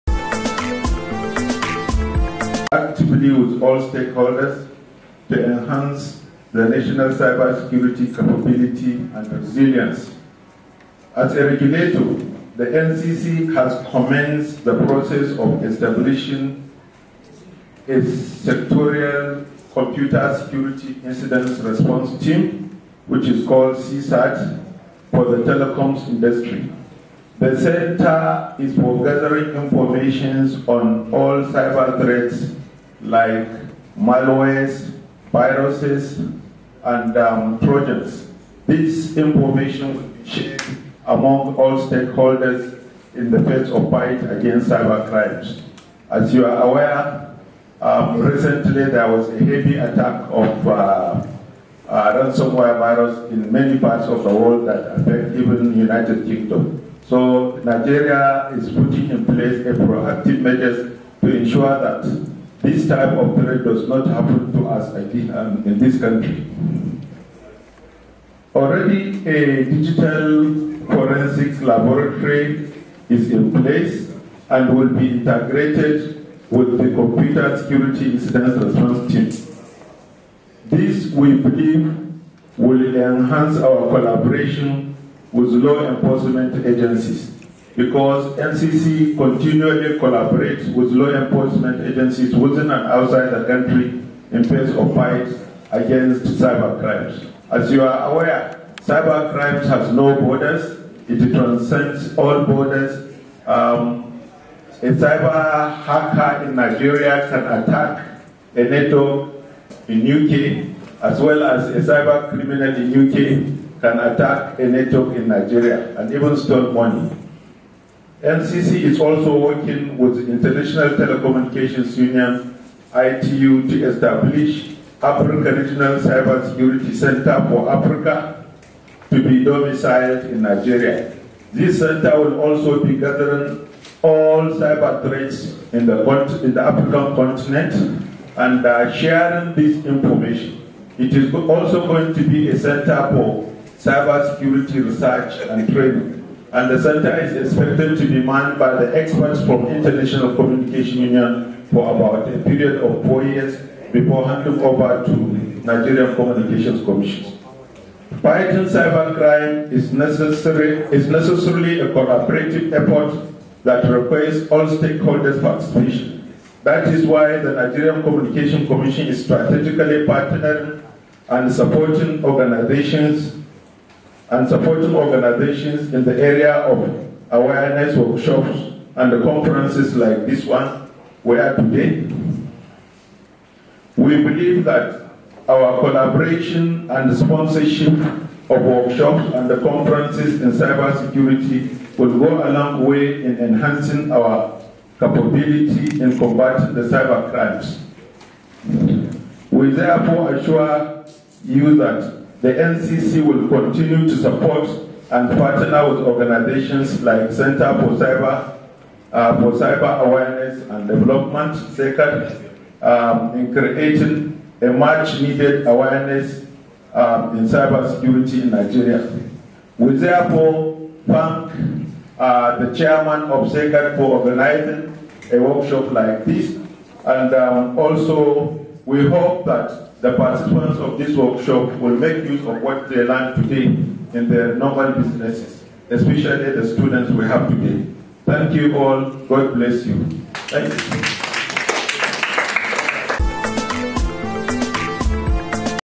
NCC TO TACKLE CYBER THREAT
SPEECH OF THE EVC OF THE NCC DURING THE WORLD SAFER INTERNET DAY IN LAGOS.